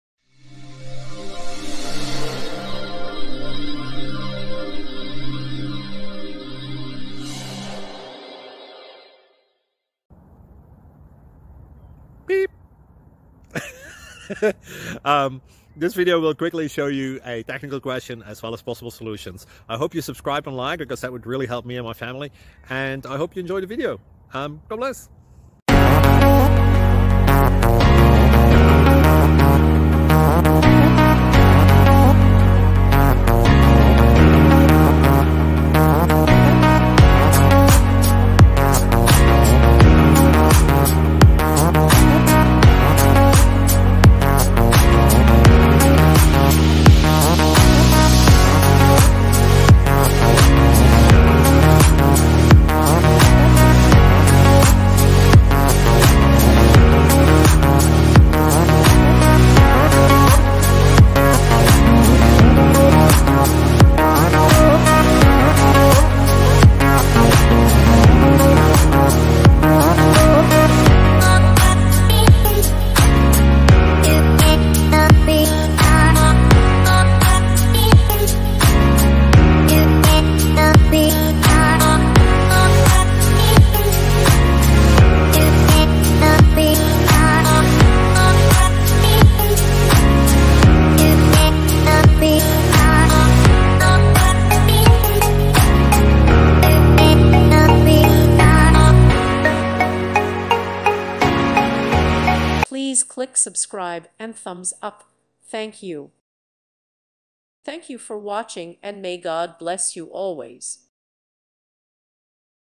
Does native async implementation in languages like Python and C# Solve the "Free Lunch is Over"...
Software Engineering: Does native async implementation in languages like Python and C# Solve the "Free Lunch is Over" problem?

With thanks & praise to God, and with thanks to the many people who have made this project possible! You are responsible for your own actions.